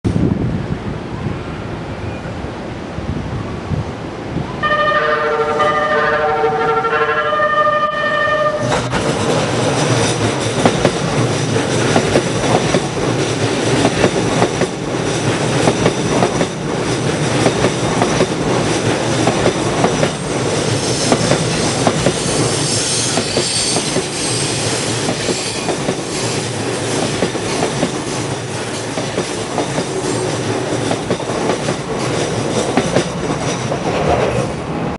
JR成田・総武本線 E259系 特急「成田エクスプレス(N`EX)」 千葉通過③
2010年8月1日撮影
Canon(キヤノン) EOS Kiss X4使用

ミュージックホーン付きです。